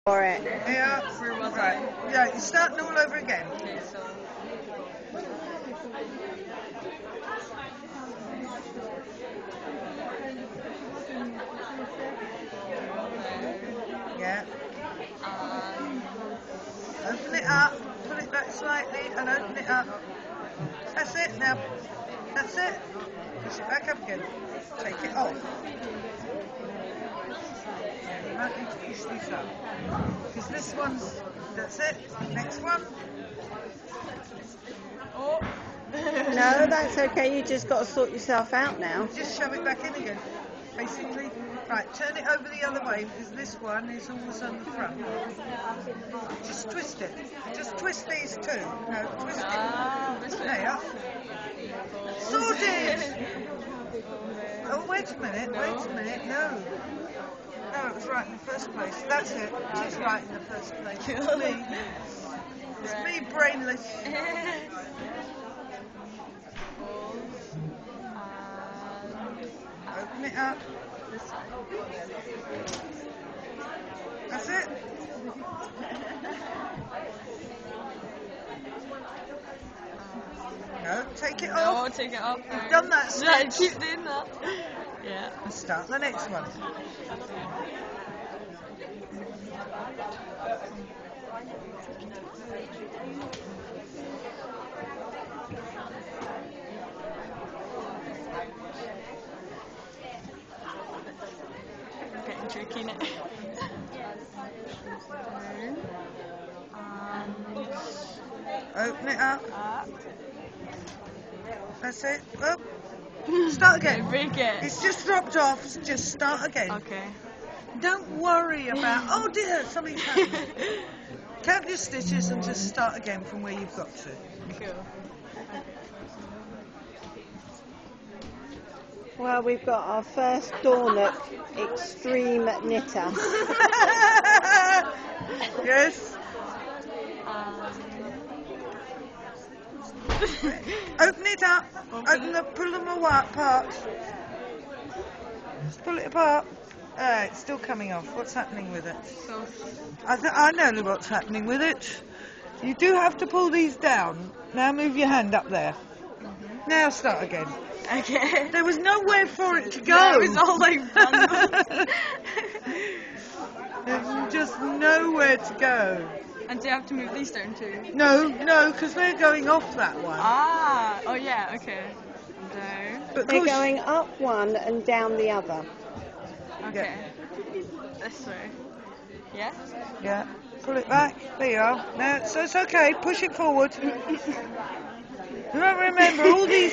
Beginner Extreme Knitting
This is Hazel Murray at Dornoch Social Club learning how to knit. She is a student from Golspie High School who hopes to put the skills she has developed to use in her aspiration to further education starting with Art and Design at college then specialising in either Fashion Design or Product design.